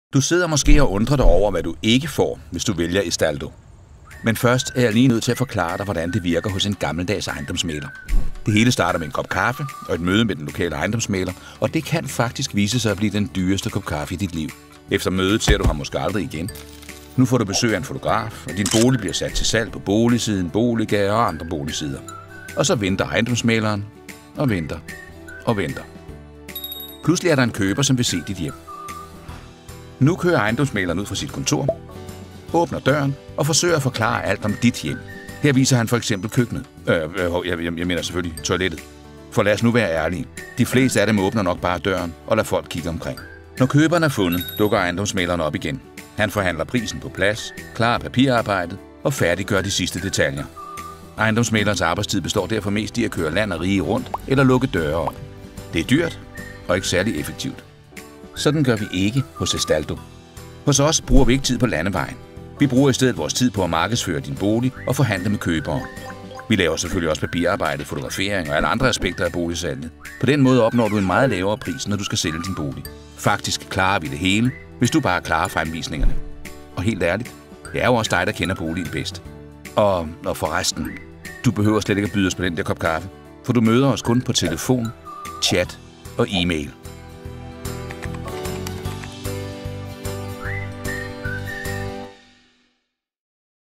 Hvordan virker Estaldo? 🏡
Vores ejendomsmægler klarer alt papirarbejdet, du klarer selv fremvisningerne. Det synes vi er meget smartere end den traditionelle ejendoms­mægler og langt mere sikkert end selvsalg.

Du kan også sælge gennem Estaldo - Digital ejendomsmægler.
Til et fast salær på blot 12.500,- kroner.

Kontakt os på Estaldo.com

Estaldo.com
Danmarks digitale ejendomsmægler